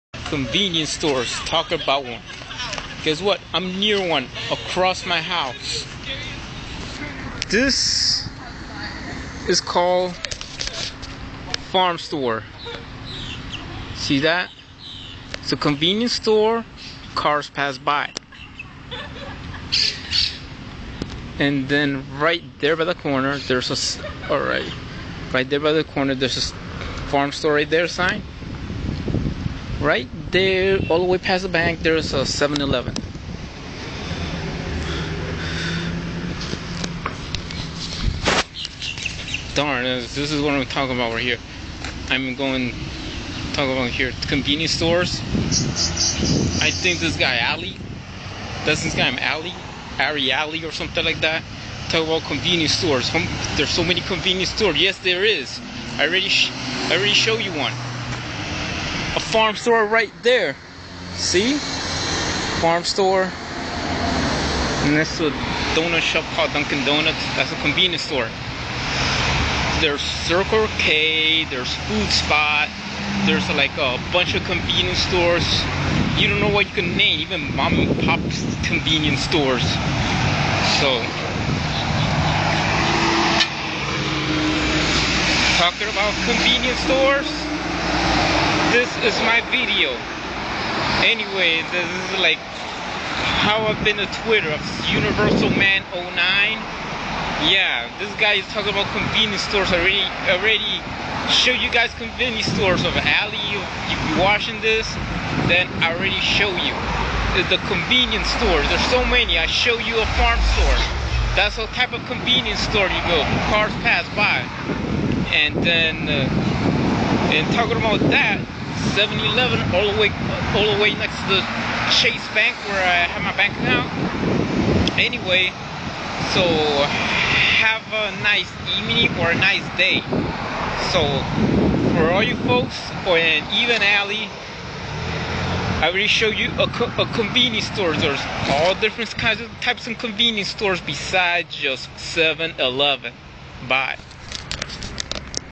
Convenience Stores
This dude Ali has told me about convienience stores and how many they are well I said shitloads on my UniversalMan09 twitter account and I did this video about convenience stores that they are more than just 7 Eleven even though 7 Eleven is a Japanese based company but these are the convenience stores around the world I show him and everyone Farm Stores this is a drive thru convenience store  and their is a list I'll show you.
Food Spot
Circle K
WaWa
Five and Dime(Maybe the first conveniece store in the world)
7 Eleven
Farm Stores (The first drive thru convieneice store)
Quickie
and probably more including the mom and pop shops !

Hint : another name for those stores are called candy stores